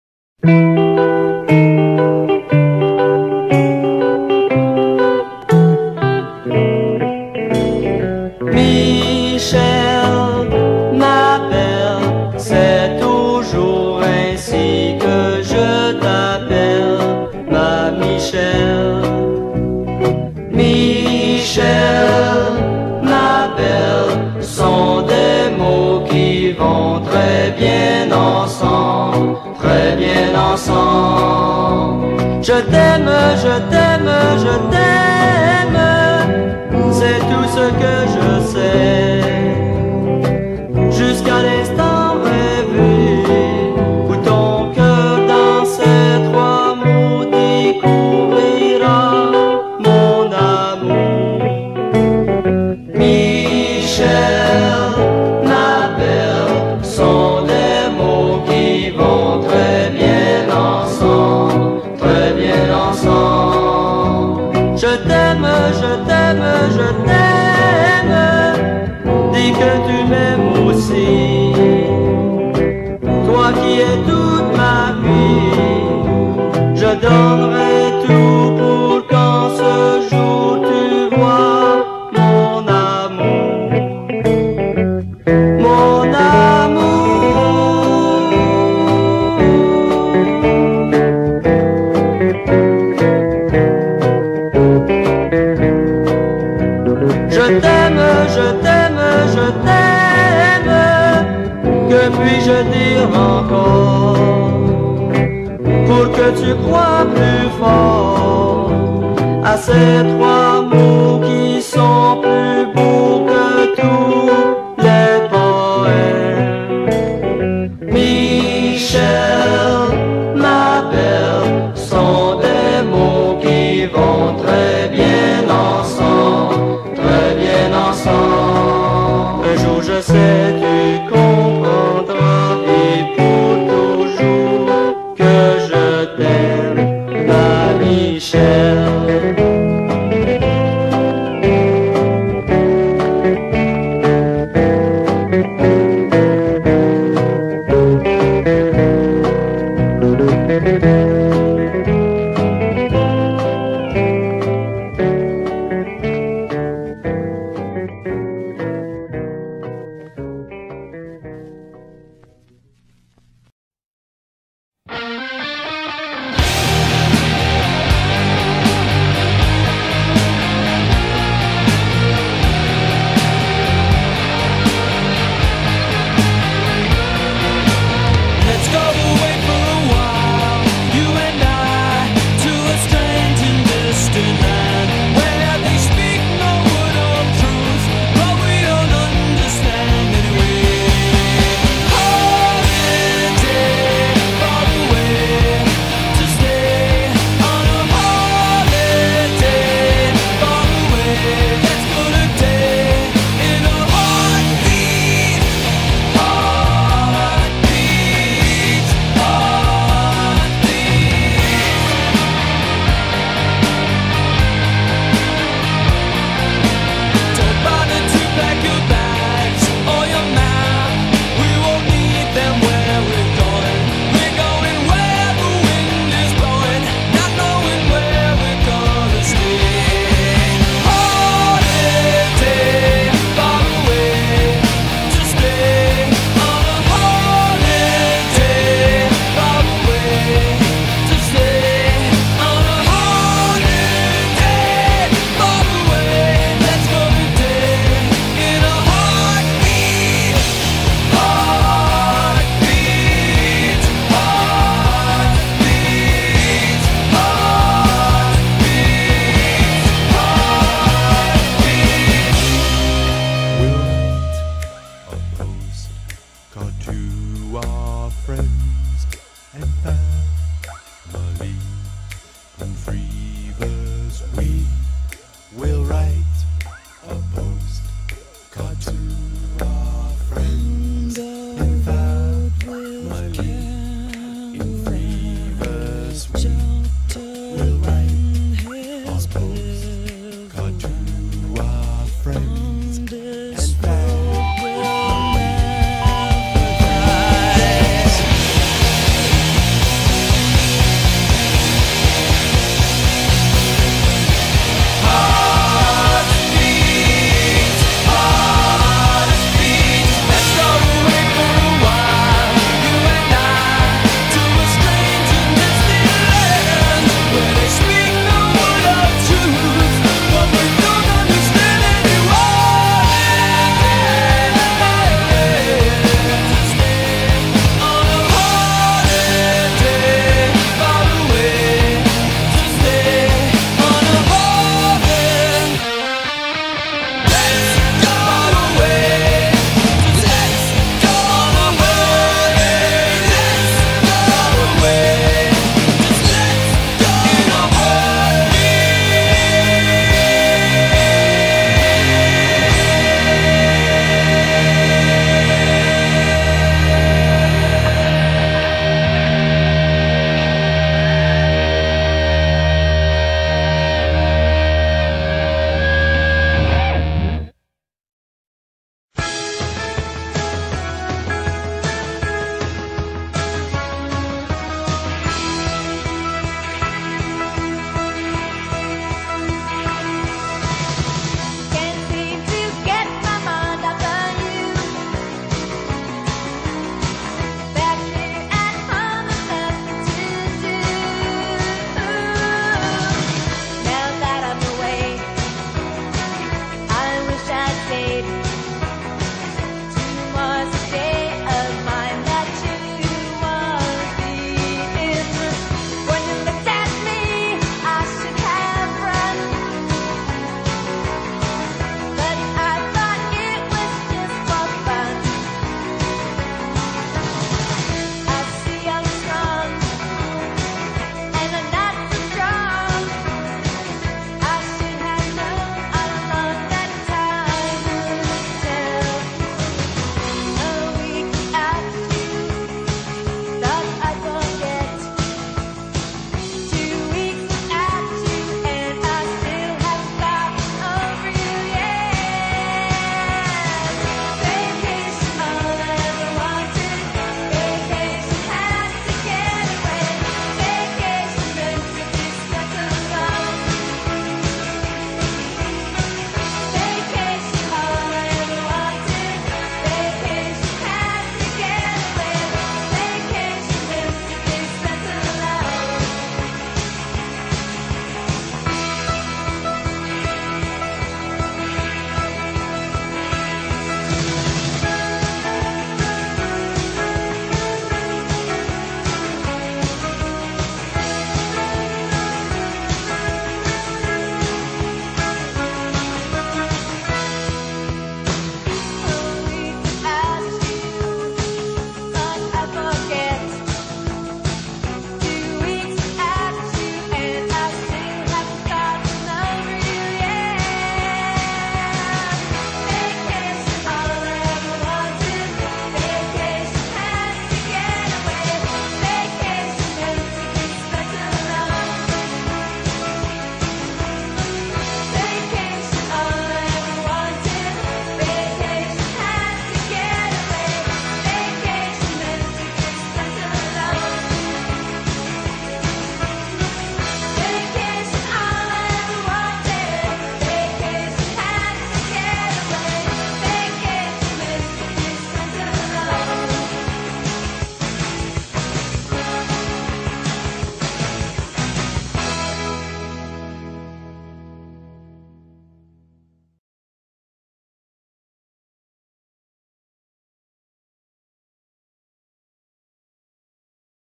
My Fall Vacation......Northwest France Photographs
Remember those old slideshows your parents used to make other people watch that meant a lot to you, but not much to anyone outside your immediate family. Well hearken back to those days of yore while I bore you to tears with my photos. Actually they kind of mean a lot to me. and they may not to you, but you can't turn away from the mesmerizing beauty of what I have captured with my Smartphone.